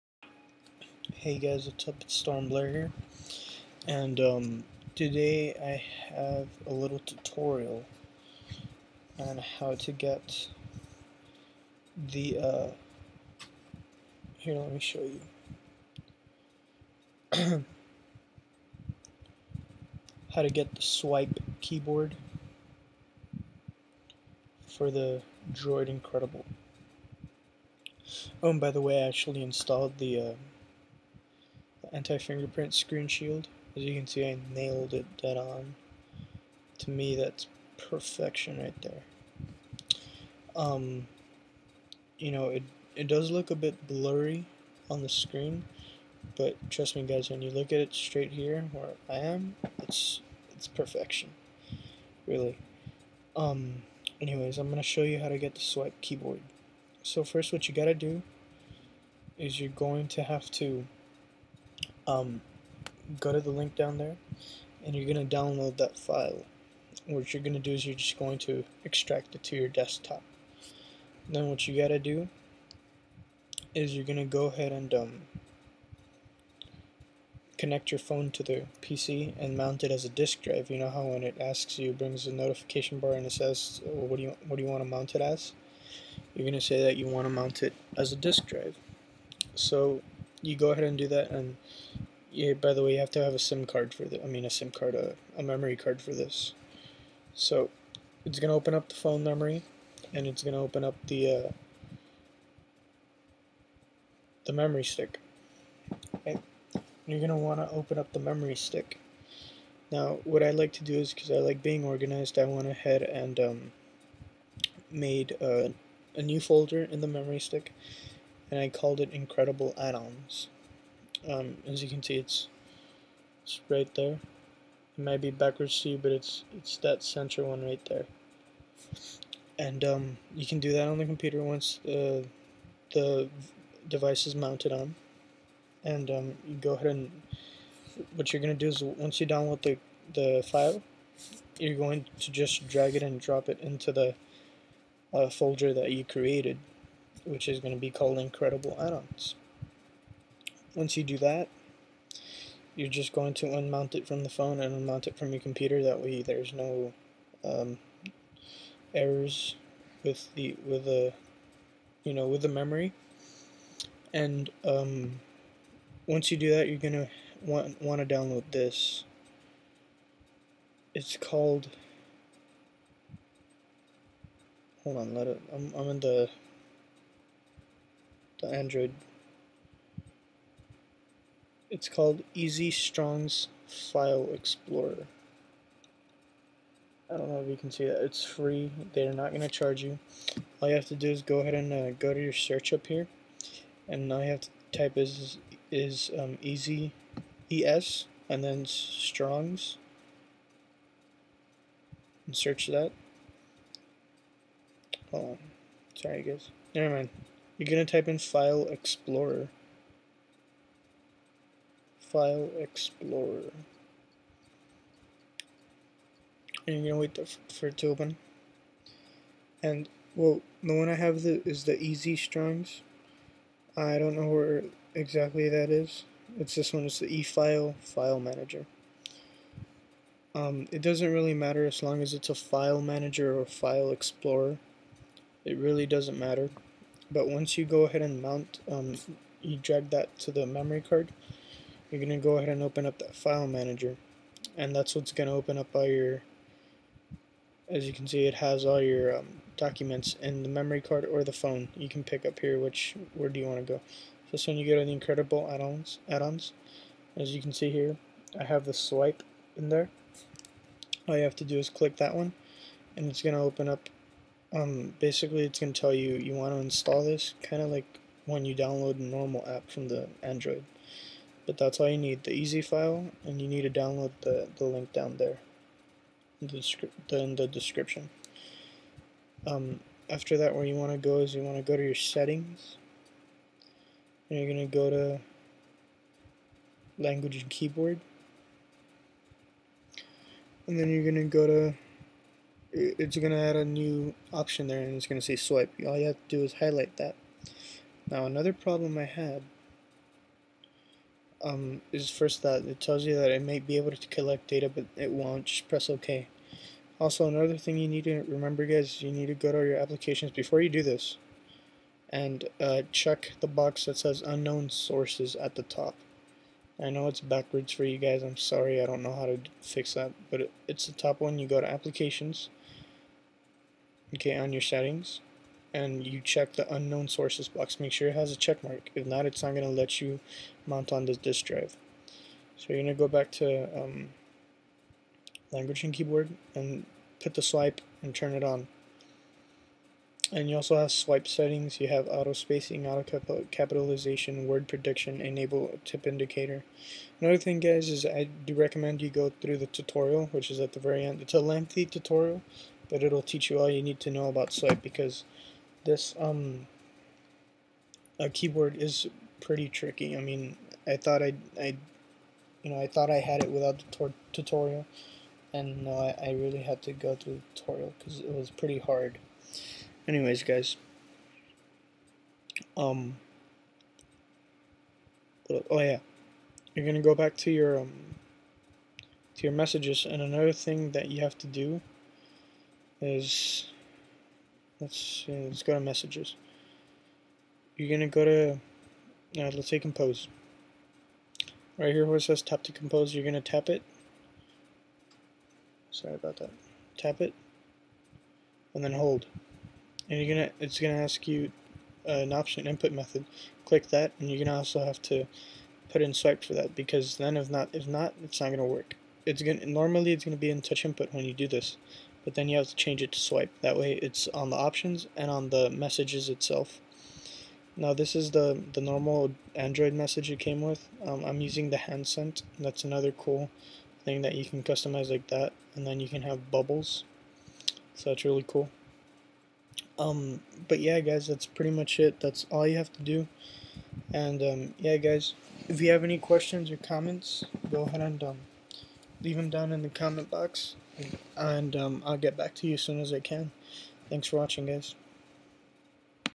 How to get swype for the Droid Incredible
here is the file you need for the swype keyboard

once downloaded you have to drag it into your sd card and get the file manager such as es file explorer you can find one in the android market for free. once you install it from the file manager go ahead and go to settings - language and keyboard - and activate the swype keyboard any questions or problems just comment or send me a message i will get back to you as soon as possible